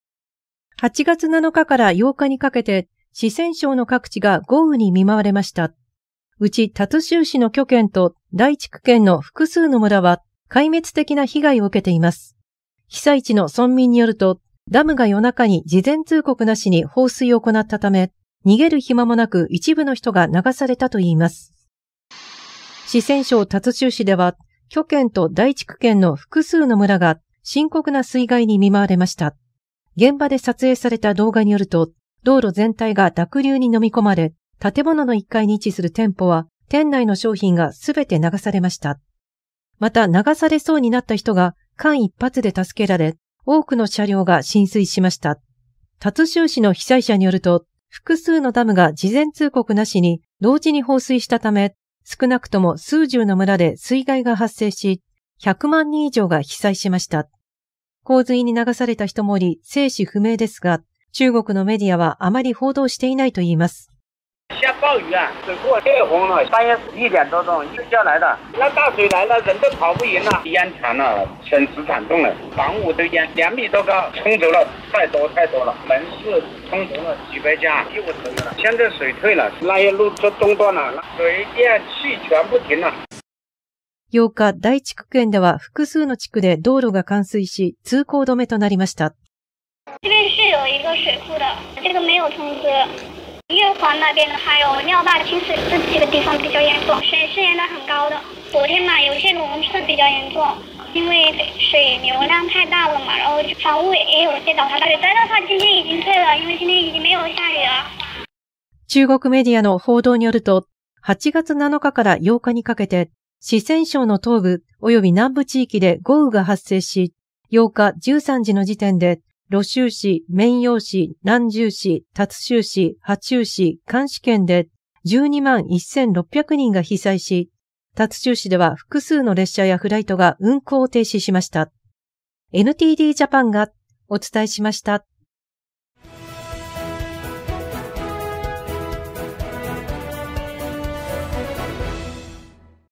四川省で豪雨 またもや通告なしの放水で深刻な水害
【新唐人NTDTV＝米NYに本部を置く衛星テレビ局 】８月7日から８日にかけて、四川省の各地が豪雨に見舞われました。うち、達州市の渠県と大竹県の複数の村は壊滅的な被害を受けています。被災地の村民によると、ダムが夜中に事前通告なしに放水を行ったため、逃げる暇もなく、一部の人が流されたといいます。

預金種目：普通預金
口座番号：2158157
口座名義：ﾄｸﾋ)ｴﾎﾟｯｸﾒﾃﾞｨｱ